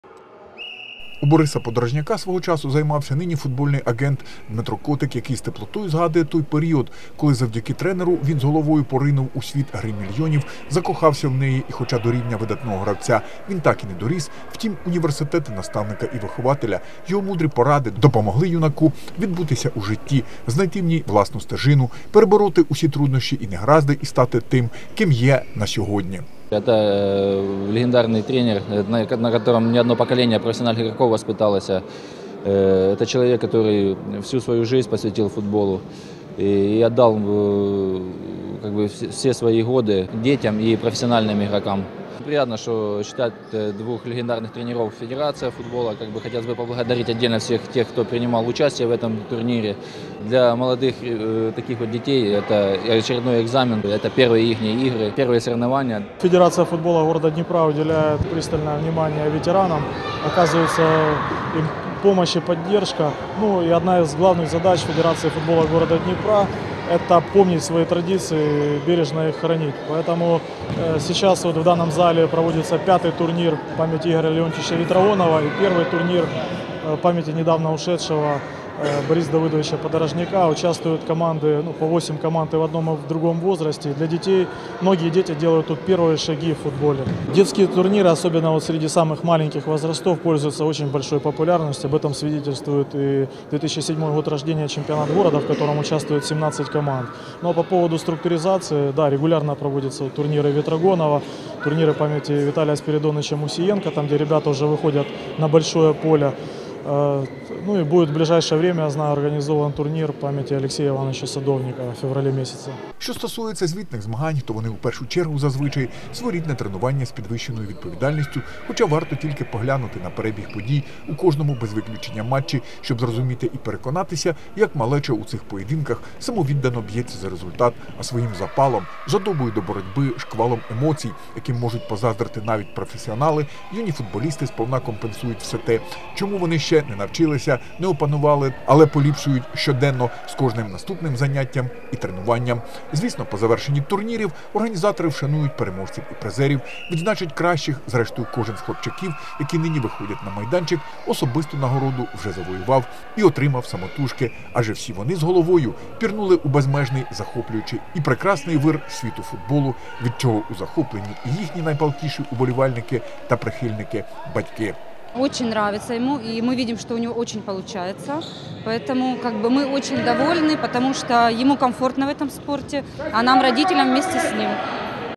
Пам'яті великих наставників і вихователів
Нині на теренах міста у розпалі два дитячо-юнацькі турніри серед наймолодших  гравців. Змагання свого часу заснувала і проводить Федерація футболу Дніпра вшановуючи у такий спосіб імена заслужених тренерів України Ігоря Вєтрогонова і Бориса Подорожняка, які своєю роботою залишили по собі яскравий слід в історії регіонального і вітчизняного футболу.